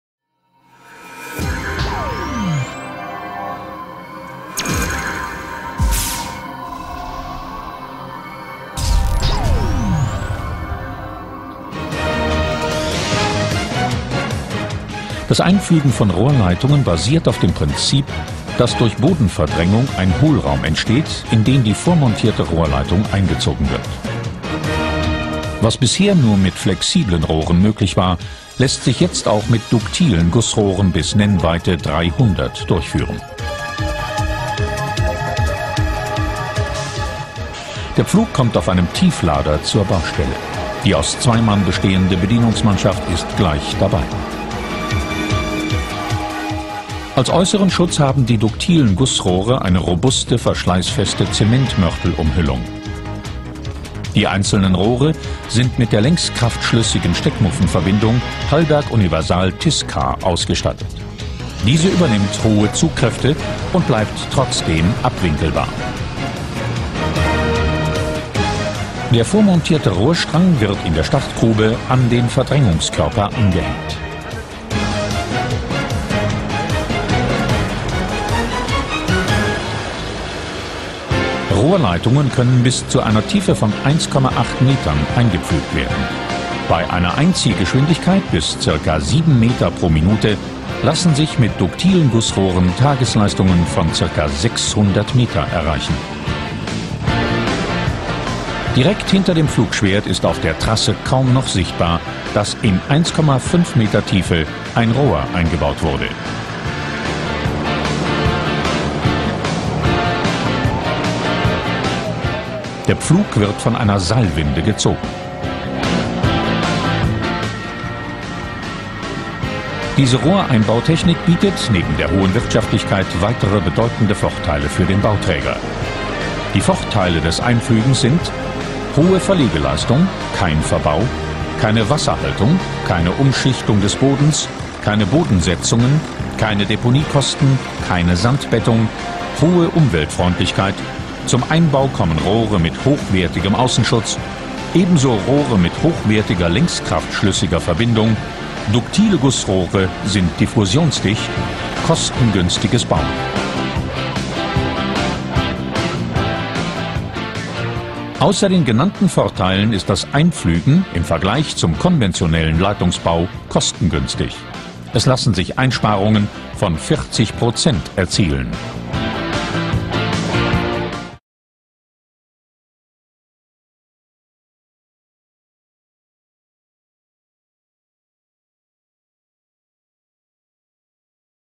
Raketenpflug beim einplügen einer Gussleitung | Frank Föckersperger GmbH
Einpflügen von starren Leitungen (Stahlleitung, Gussleitung) mit dem Föckersperger Raketenpflug